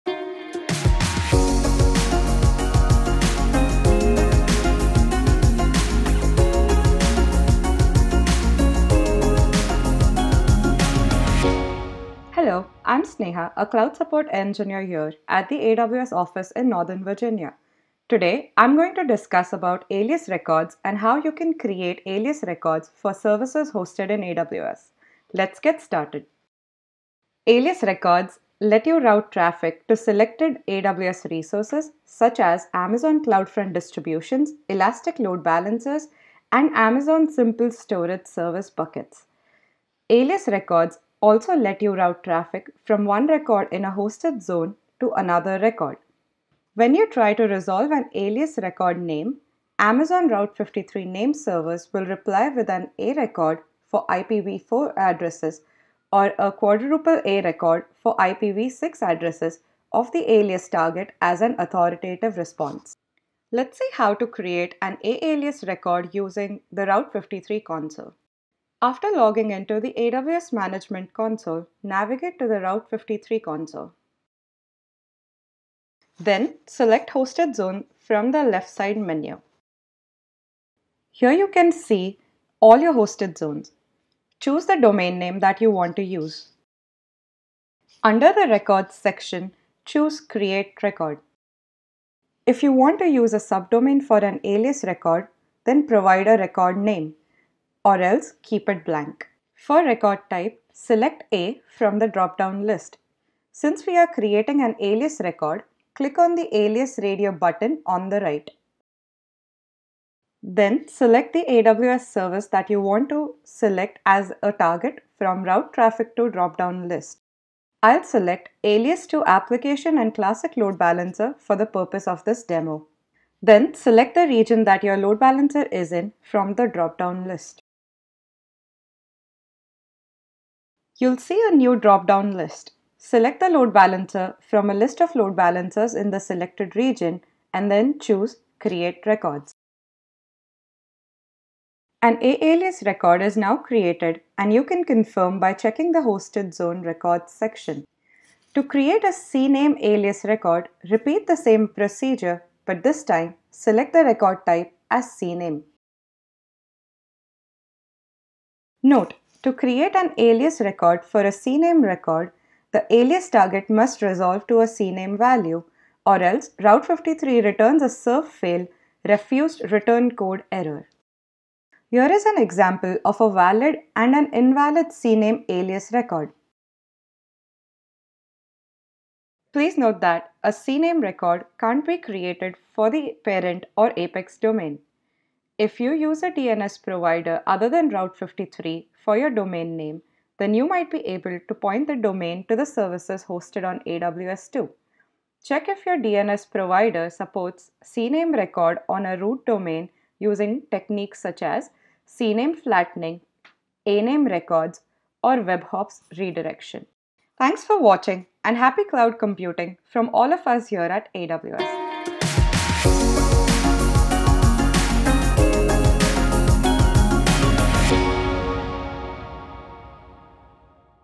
How do I create alias records for services hosted in AWS?
Skip directly to the demo: 0:26
For more details see the Knowledge Center article with this video
Sneha shows you how to create alias records for services hosted in AWS.

0:26 Introduction

1:04 Chapter 1

2:39 Chapter 2

3:41 Closing